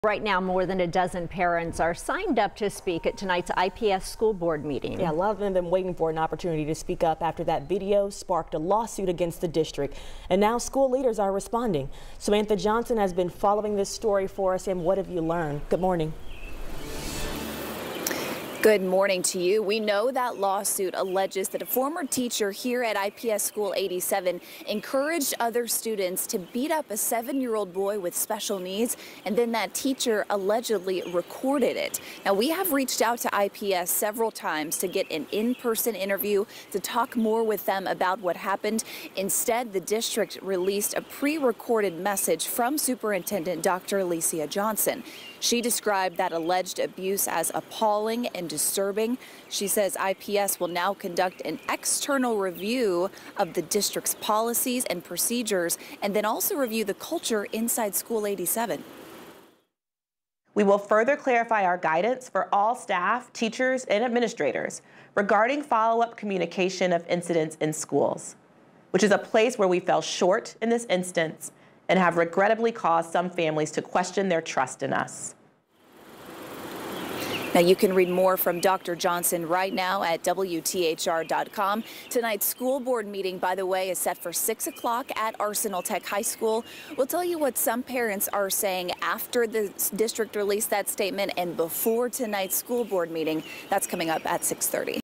'Appalling and disturbing' | IPS superintendent responds to alleged attack at elementary school
The incident, allegedly recorded by the teacher, happened at George Washington Carver Montessori IPS School 87.